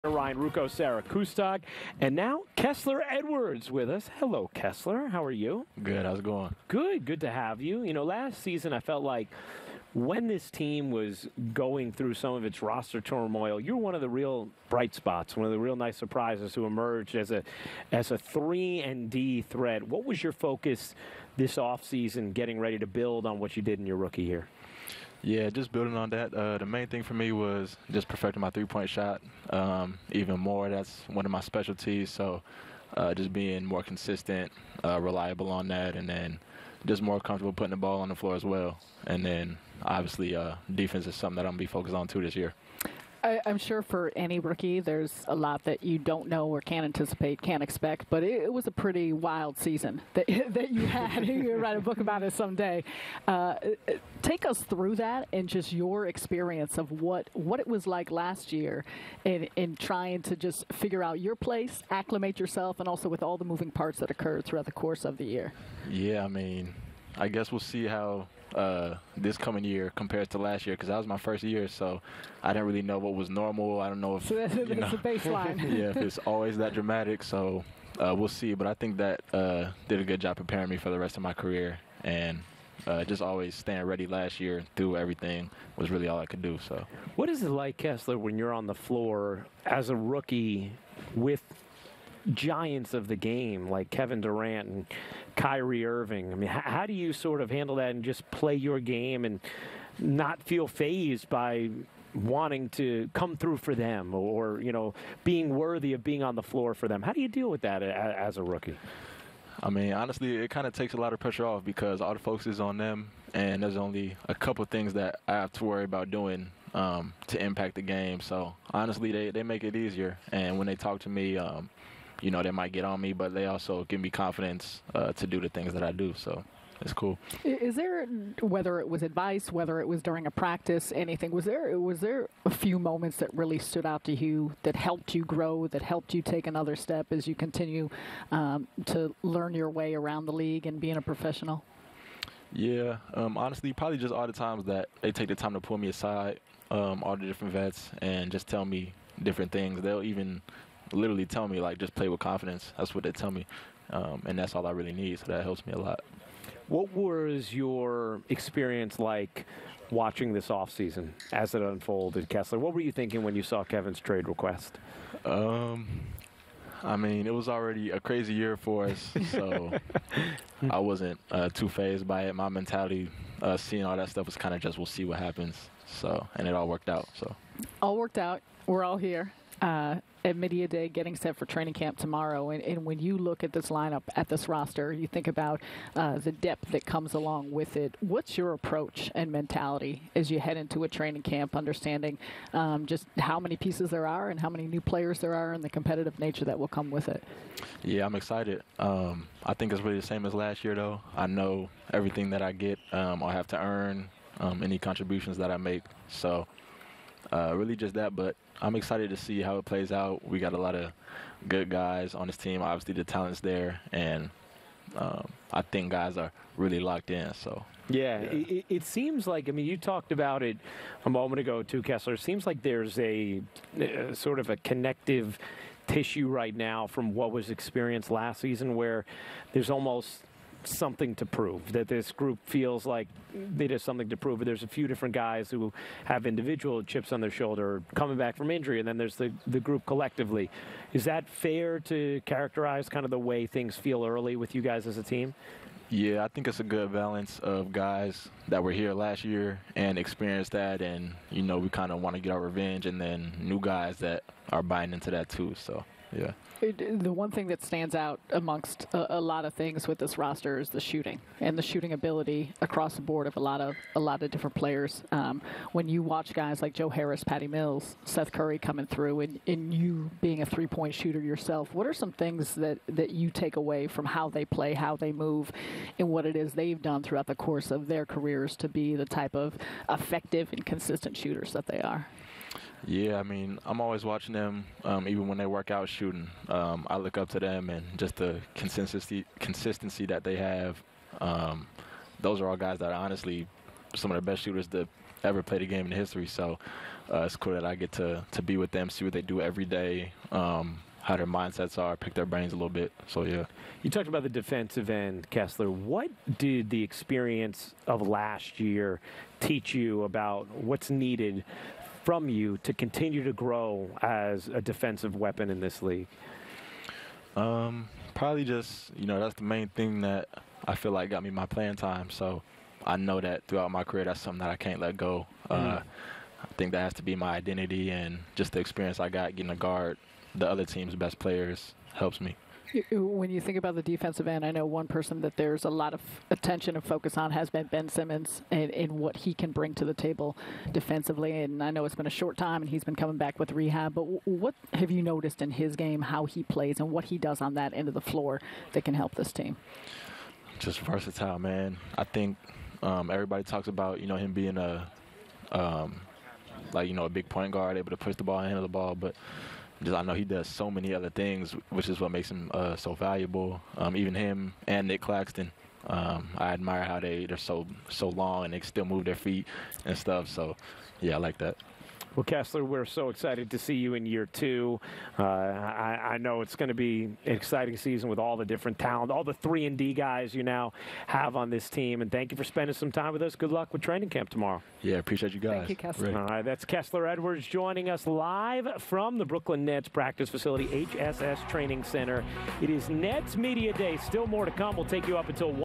Kessler Edwards interview on Brooklyn Nets Media Day 2022
Kessler Edwards discusses his rookie season, his offseason and workout plan, the Nets' offseason, growing in the NBA, and more.